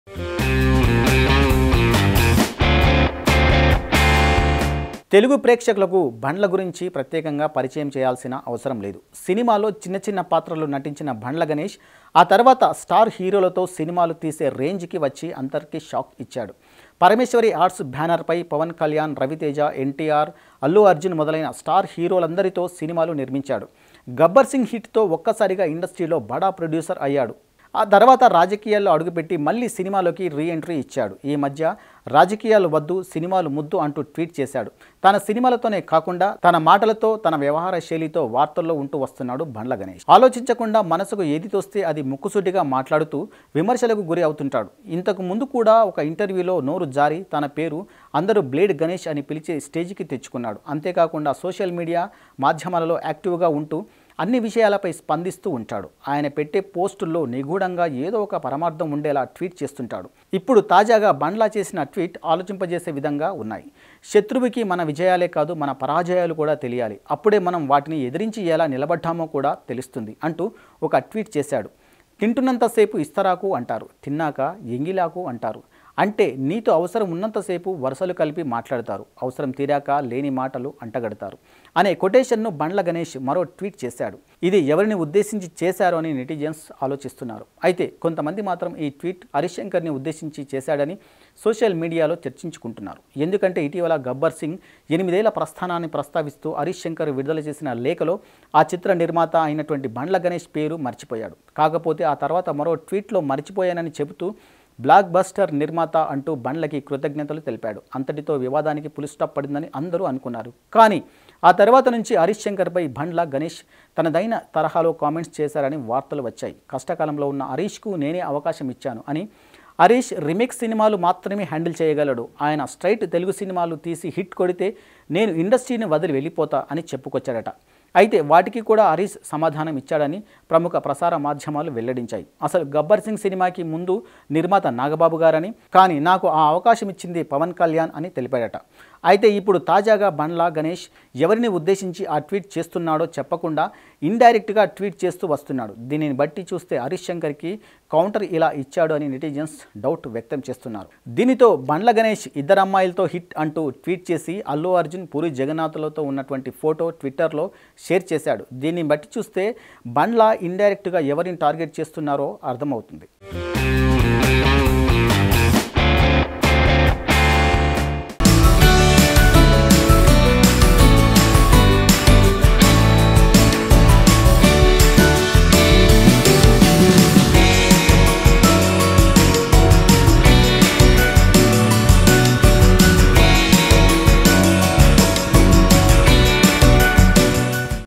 Bandla Ganesh satirical Tweet On Director Harish Shankar | Tollywood Gossips | Multiplex
Bandla Ganesh satirical Tweet On Director Harish Shankar  Tollywood Gossips  Multiplexx